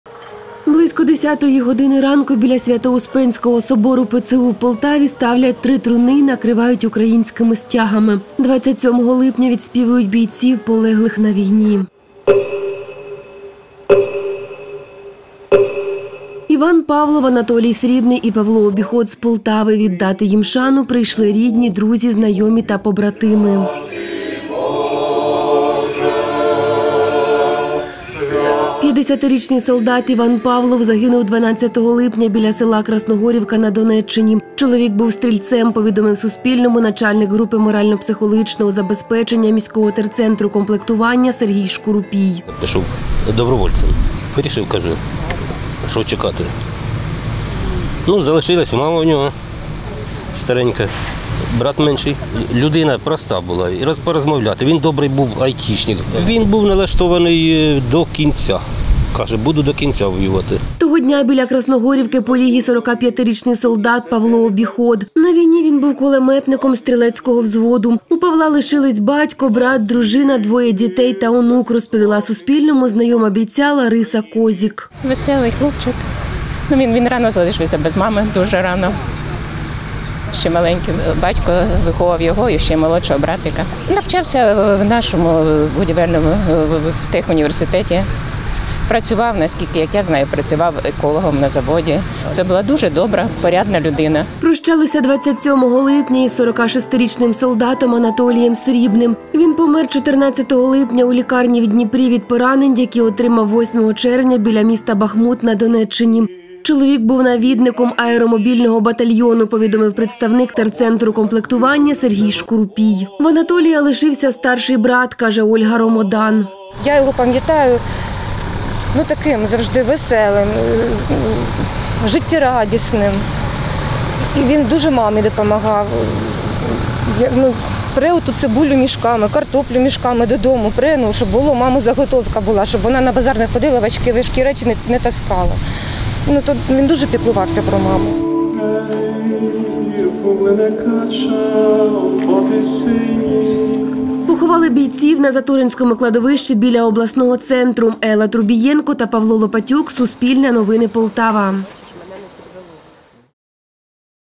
У Полтаві попрощалися із Іваном Павловим, Анатолієм Срібним і Павлом Обіходом: спогади знайомих
У Полтаві 27 липня попрощалися з Іваном Павловим, Анатолієм Срібним і Павлом Обіходом. Солдати проживали в обласному центрі, а загинули на Донеччині, повідомив начальник групи морально-психологічного забезпечення міського терцентру комплектування Сергій Шкурупій.